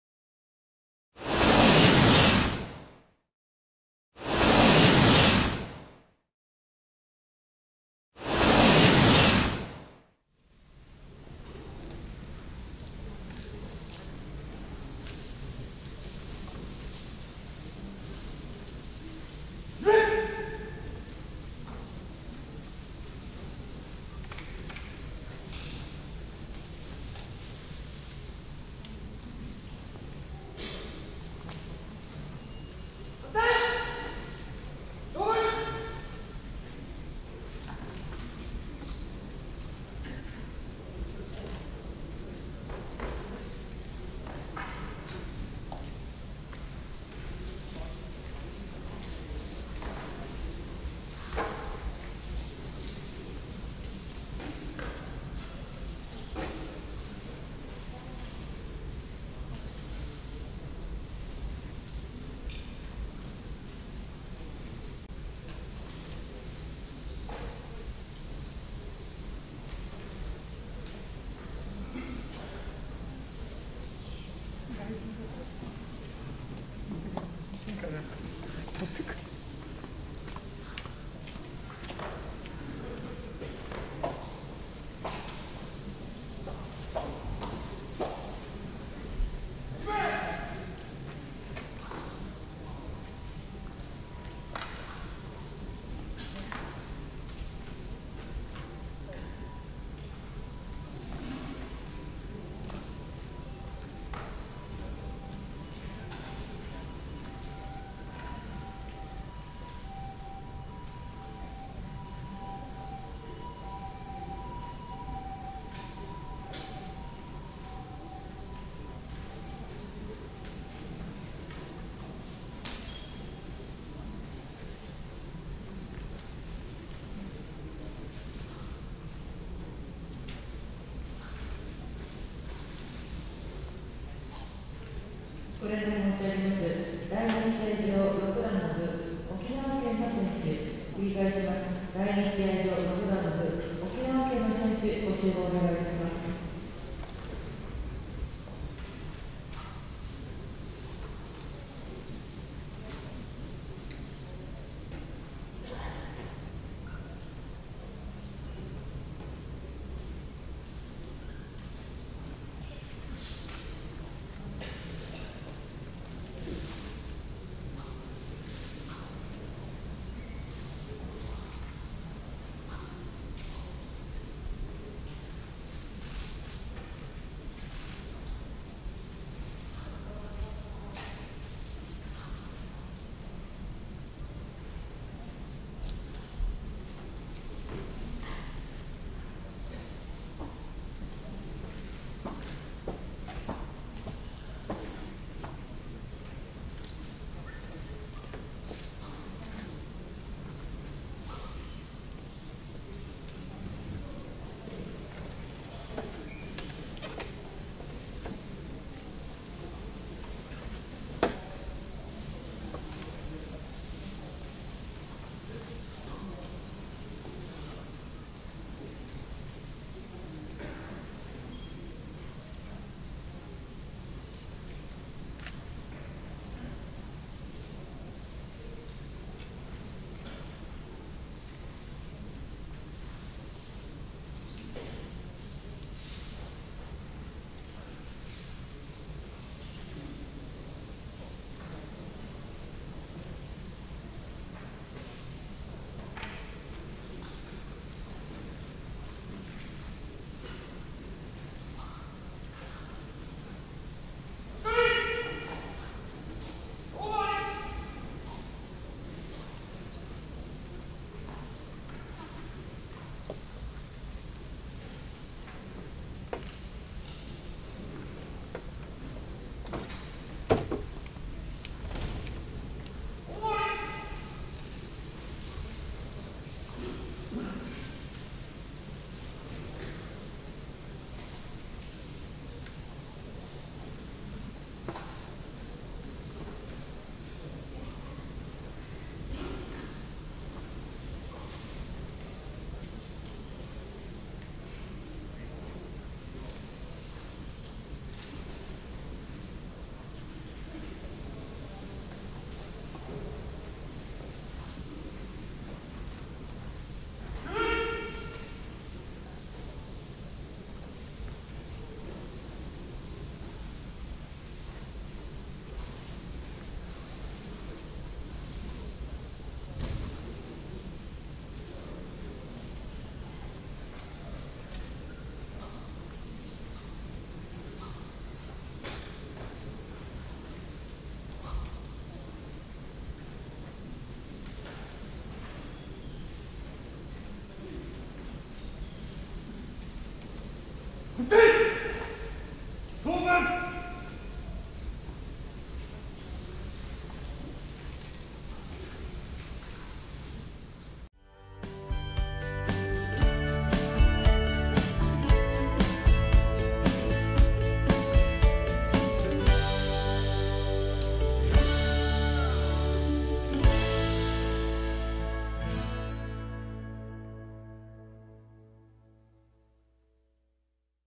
畑中 健佑 3 - 0 リチャード ストーネル -第56回 全日本居合道大会 五段の部 二回戦 28試合
第56回 全日本居合道大会  五段の部 二回戦
期日：2021年10月９日（土）
会場：東京武道館
The 56th All Japan IAIDO TAIKAI will be held at Tokyo Budokan on 9th October 2021. 5 Dan Second Round

[赤](長崎)畑中 健佑 3 - 0 リチャード ストーネル(兵庫)[白]
[Red](Nagasaki)Kensuke HATANAKA 3 - 0 Sutoneru RICHADO (Hyogo)[White]